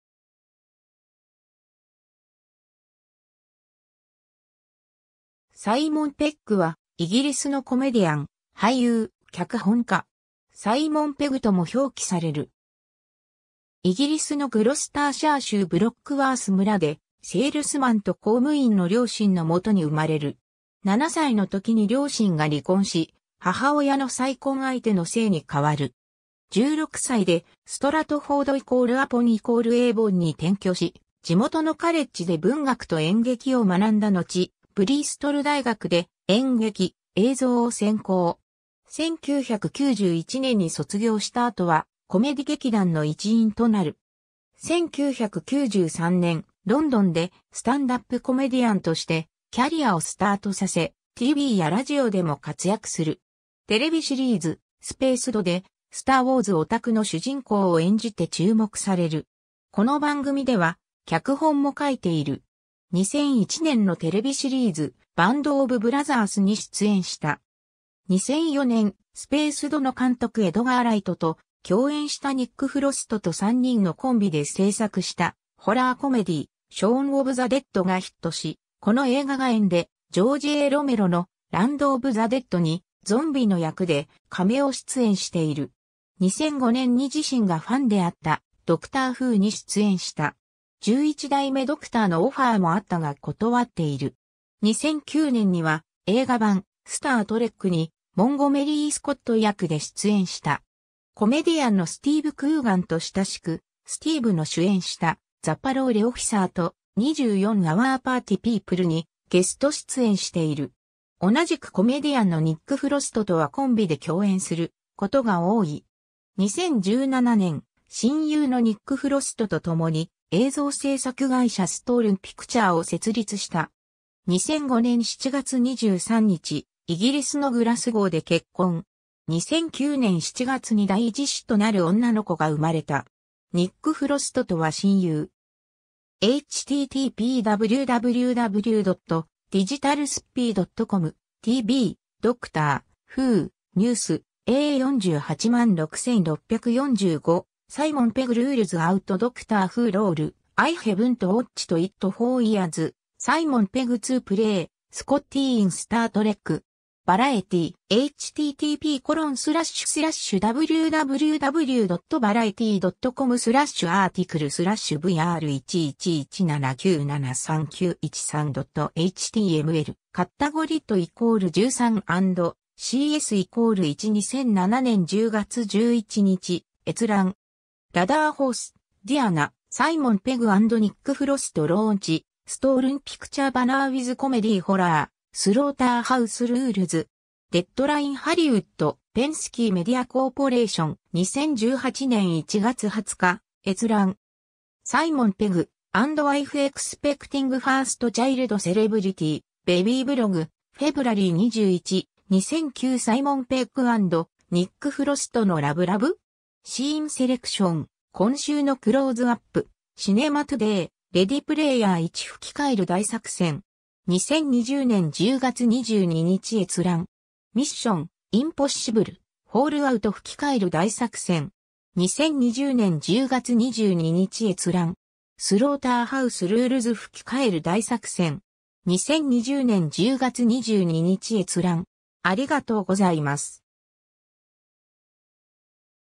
サイモン・ペッグ
サイモン・ペッグ（Simon Pegg, 1970年2月14日 - ）は、イギリスのコメディアン、俳優、脚本家。
サイモン・ペグとも表記される。
 イギリスのグロスターシャー州ブロックワース村で、セールスマンと公務員の両親のもとに生まれる。
7歳の時に両親が離婚し、母親の再婚相手の姓に変わる。
16歳でストラトフォード＝アポン＝エイヴォンに転居し、地元のカレッジで文学と演劇を学んだのち、ブリストル大学で演劇・映像を専攻。
1991年に卒業した後は、コメディ劇団の一員となる。
 1993年、ロンドンでスタンダップ・コメディアンとしてキャリアをスタートさせ、TVやラジオでも活躍する。
テレビシリーズ『スペースド』で、『スター・ウォーズ』おたくの主人公を演じて注目される。
この番組では脚本も書いている。
 2001年のテレビシリーズ『バンド・オブ・ブラザース』に出演した。
 2004年、『スペースド』の監督エドガー・ライトと、共演したニック・フロストと3人のコンビで製作したホラー・コメディ『ショーン・オブ・ザ・デッド』がヒットし、この映画が縁でジョージ・A・ロメロの『ランド・オブ・ザ・デッド』にゾンビの役でカメオ出演している。
 2005年に自身がファンであった『ドクター・フー』に出演した。
11代目ドクターのオファーもあったが断っている。
 2009年には映画版『スター・トレック』にモンゴメリー・スコット役で出演した。
 コメディアンのスティーヴ・クーガンと親しく、スティーヴの主演した『The Parole Officer』と『24アワー・パーティ・ピープル』にゲスト出演している。
同じくコメディアンのニック・フロストとはコンビで共演することが多い。
 2017年、親友のニック・フロストとともに映像制作会社"Stolen Picture"を設立した。
 2005年7月23日、イギリスのグラスゴーで結婚。
2009年7月に第一子となる女の子が生まれた。
ニック・フロストとは親友。
^ “Simon Pegg to play Scotty in "Star Trek"”. Variety. (2007年10月12日). 2007年10月11日閲覧。
 ^ Lodderhose, Diana (2017年5月16日). “Simon Pegg & Nick Frost Launch Stolen Picture Banner With Comedy-Horror 'Slaughterhouse Rulez'”. Deadline Hollywood. Penske Media Corporation. 2018年1月20日閲覧。
 ^ Simon Pegg and Wife Expecting First Child Celebrity Baby Blog, February 21, 2009 ^ サイモン・ペッグ＆ニック・フロストのラブラブ!?シーンセレクション：今週のクローズアップ - シネマトゥデイ ^ “レディ・プレイヤー1 – Fukikaeru-Daisakusen” (日本語). 2020年10月22日閲覧。
 ^ “ミッション：インポッシブル／フォールアウト – Fukikaeru-Daisakusen” (日本語). 2020年10月22日閲覧。
 ^ “スローターハウス・ルールズ – Fukikaeru-Daisakusen” (日本語). 2020年10月22日閲覧。